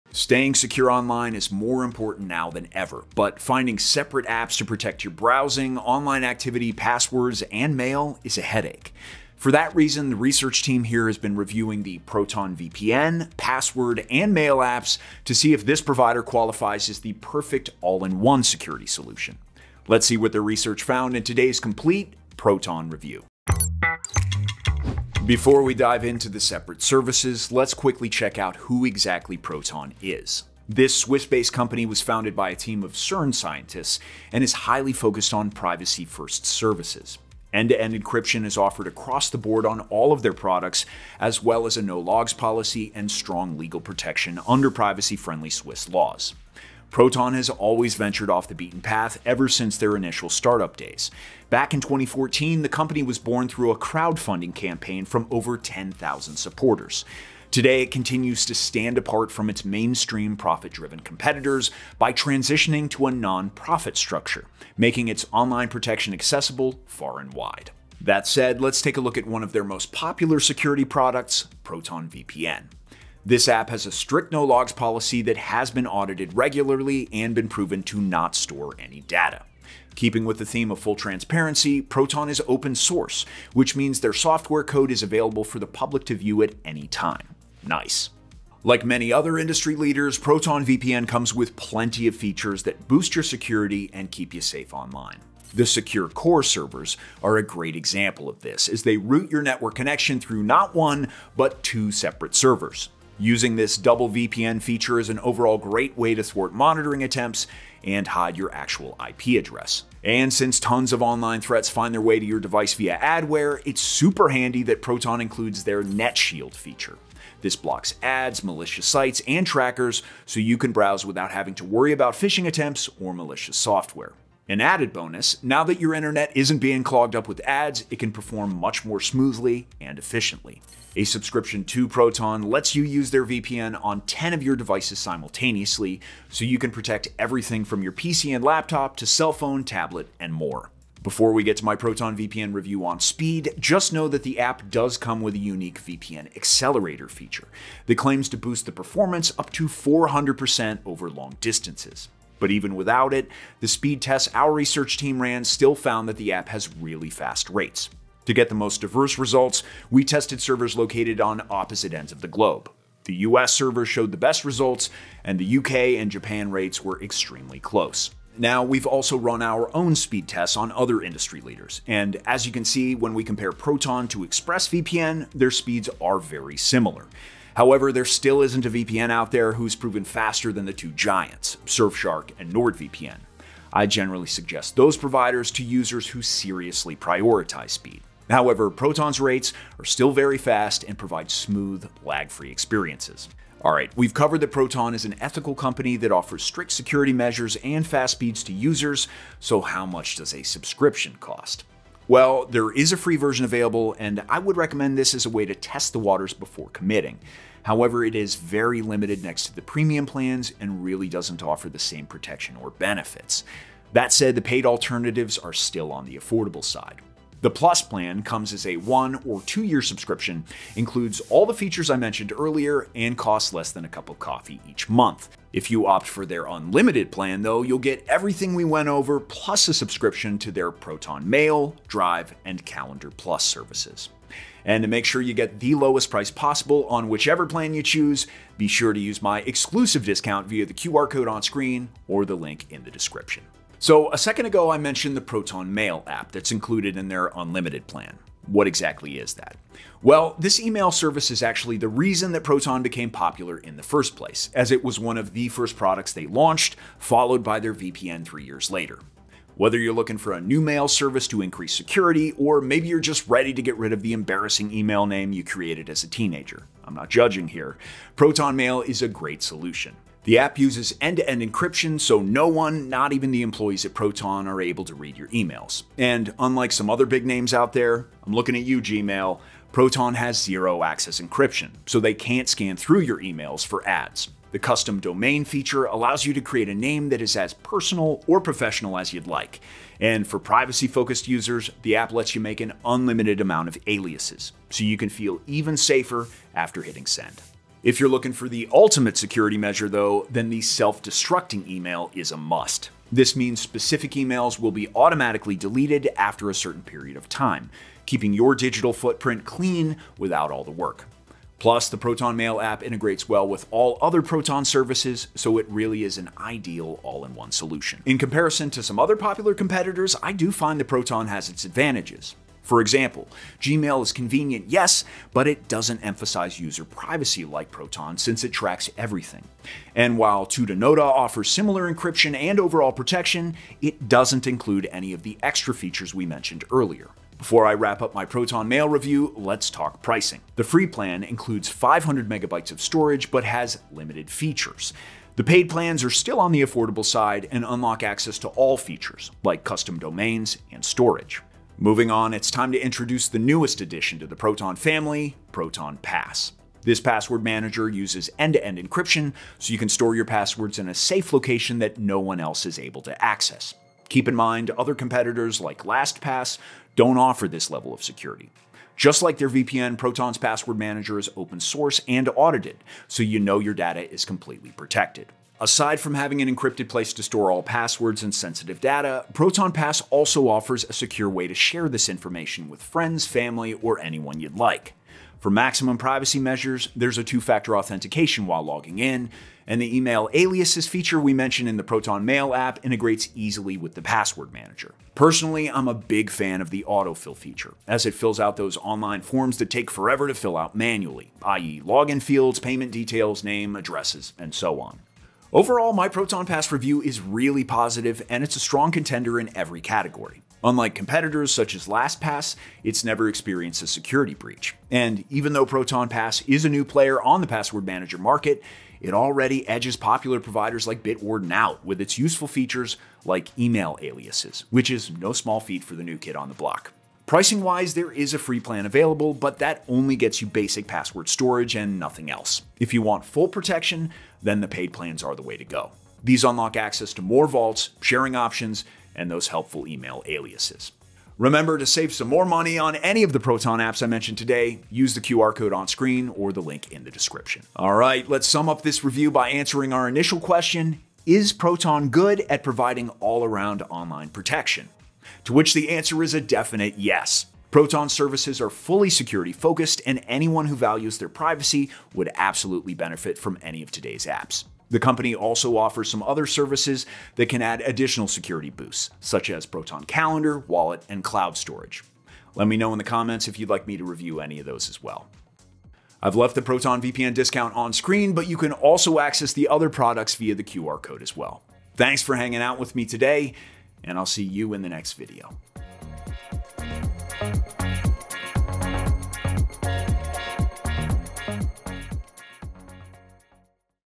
Ultimate Proton Privacy review 2026 | Proton VPN + Proton Mail + Proton Pass
✨ Get the ultimate Proton deal, save money  and be protected online! ✨

In this video, I review all Proton products, including Proton VPN, Mail, and Pass, to see if they’re worth our money in 2026.

00:00 Intro
00:25 Is Proton trustworthy?
01:08 Proton VPN review. Is Proton the best VPN?
04:04 Proton Mail review
05:51 Proton Pass review
07:27 Is Proton worth your money?